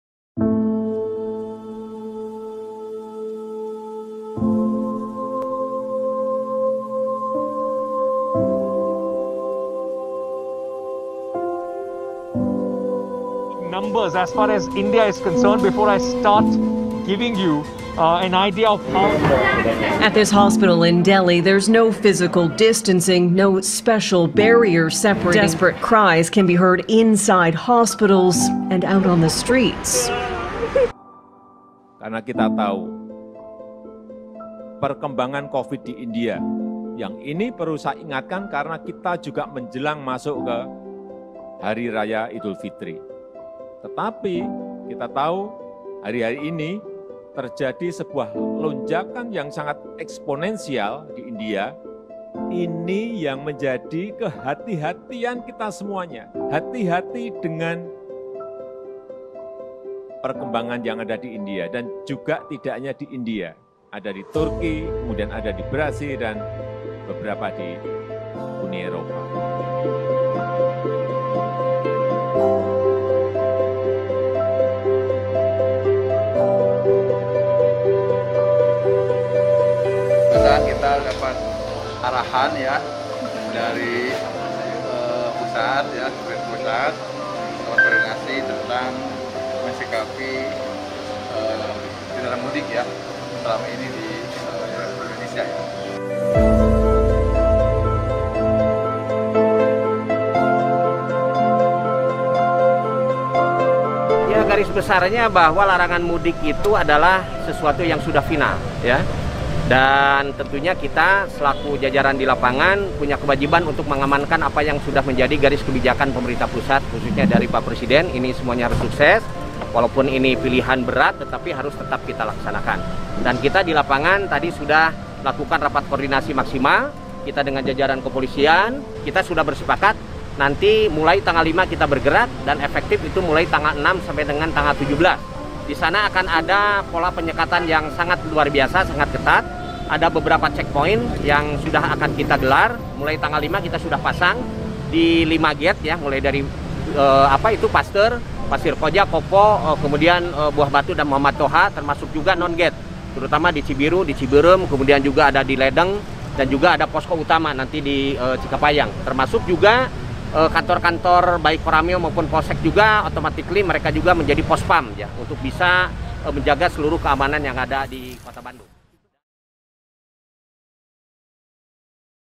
Tak Perlu Mudik, Mudik Sebabkan Penyebaran COVID-19 Tak Terkendali
Covid-19 di India tak terkendali, pasien membludak dan tanpa perawatan. Terjadi karena lengahnya masyarakat dan rendahnya kewaspadaan.

Wargi Bandung, hayu disiplin dan taati aturan dengan tidak mudik. Semua rindu, semua ingin bertemu, tapi Covid-19 masih ada di dekat kita dan harus kita cegah untuk kebaikan diri dan keluarga.

Semoga upaya dan kesabaran kita menjadi buah yang baik untuk menekan penyebaran Covid-19. Jadikan Ramadan ini penuh berkah dengan keamanan dan mengutamakan kesehatan keluarga 🙏